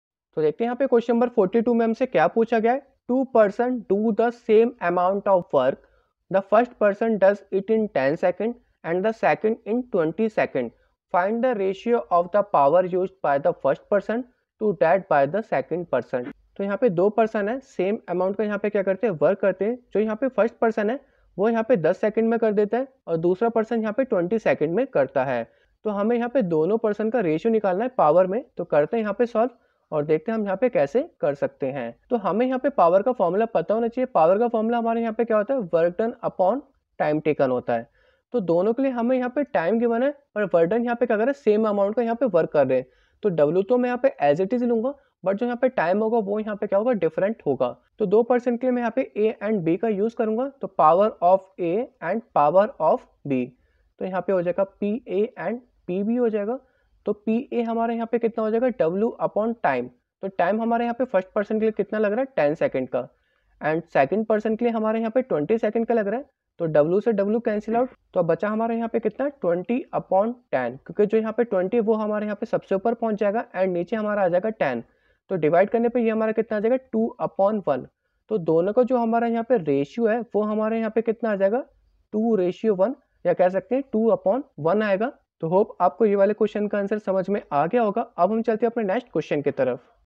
Two persons do the same amount of work. The first person does it in 10 s and the second in 20 s.
Q.42 Two persons do the same amount of work. The first person does it in 10 s and the second, in 20 s. Find the ratio of the power used by the first person to that by the second person.